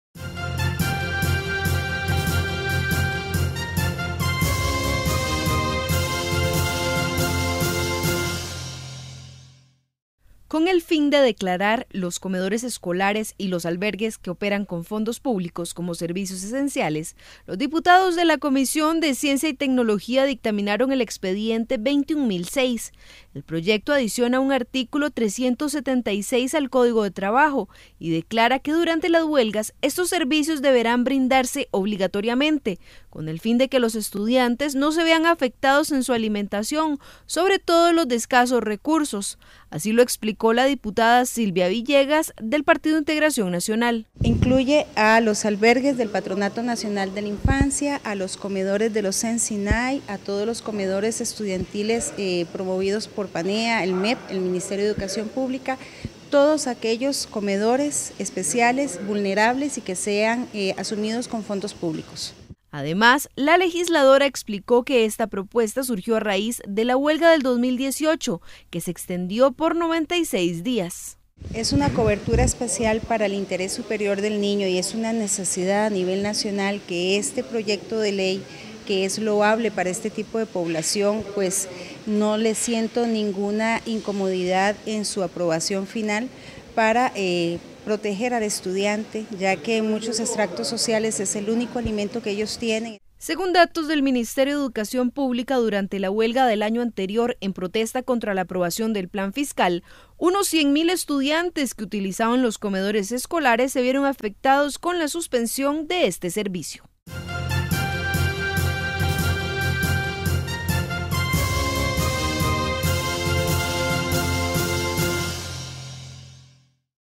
DIPUTADOS BUSCAN DECLARAR COMEDORES Y ALBERGUES ESCOLARES COMO SERVICIO ESENCIAL
Con el fin de declarar los comedores escolares y los albergues que operen con fondos públicos como servicios esenciales, los diputados de la Comisión de Ciencia y Tecnología dictaminaron el expediente 21.006